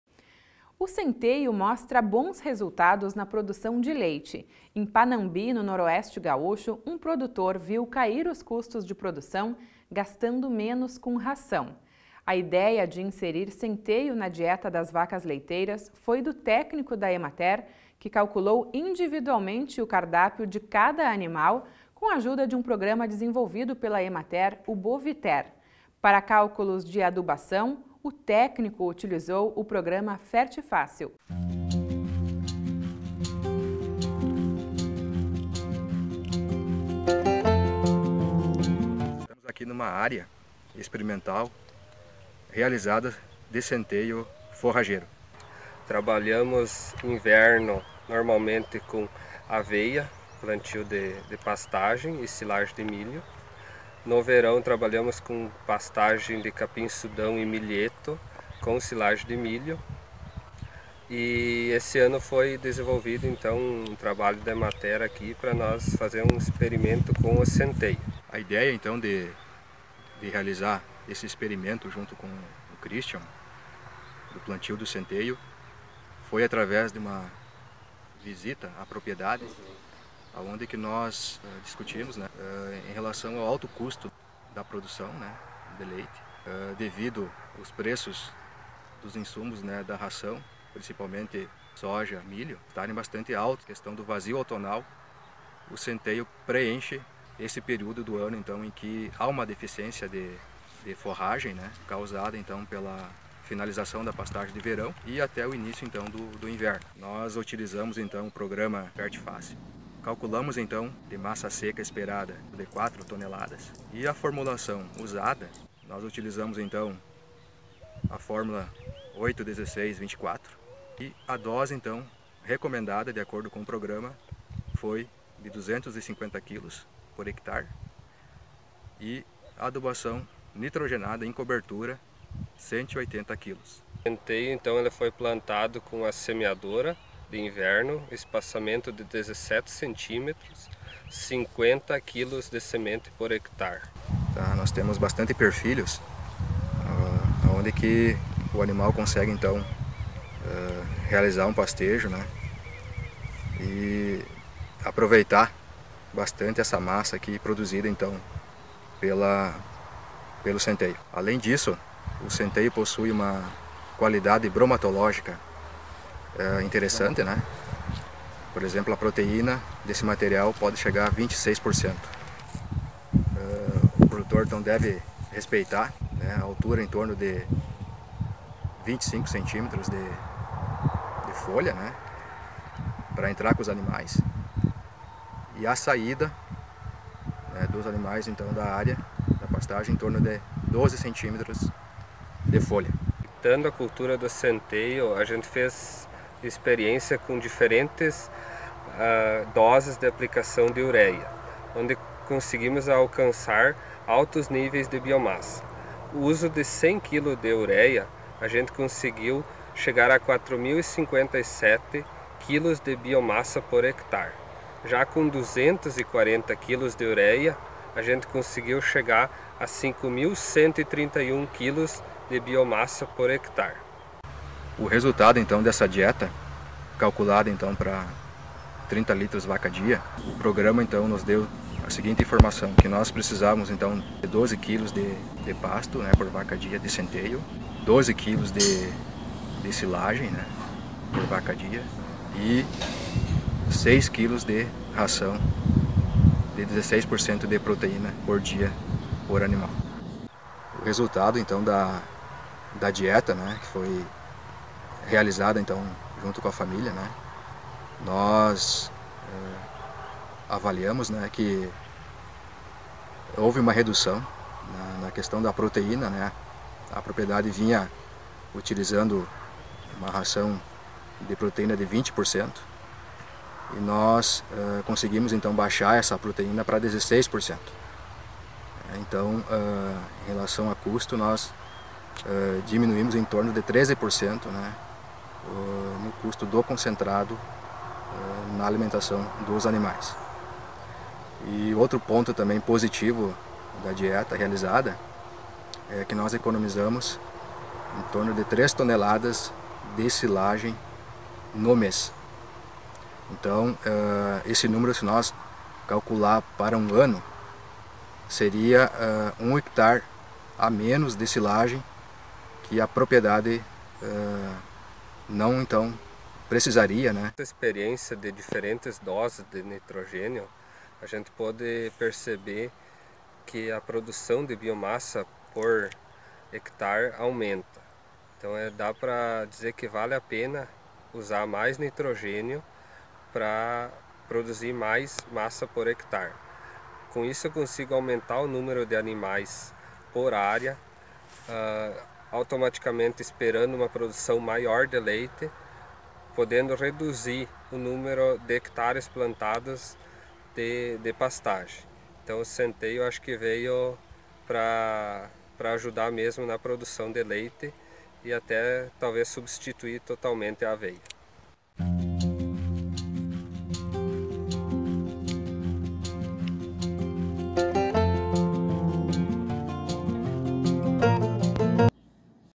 Centeio é alternativa para diminuir os custos de alimentação das vacas leiteiras - Rio Grande Rural
O centeio mostra bons resultados na produção de leite. Em Panambi, no Noroeste gaúcho, um produtor viu cair os custos de produção, gastando menos com ração. A ideia de inserir centeio na dieta das vacas leiteiras foi do técnico da Emater/RS, que calculou individualmente o cardápio de cada animal com a ajuda de um programa desenvolvido pela Emater/RS, o o BOVITER. Para cálculos de adubação, o técnico utilizou o programa FERTFÁCIL.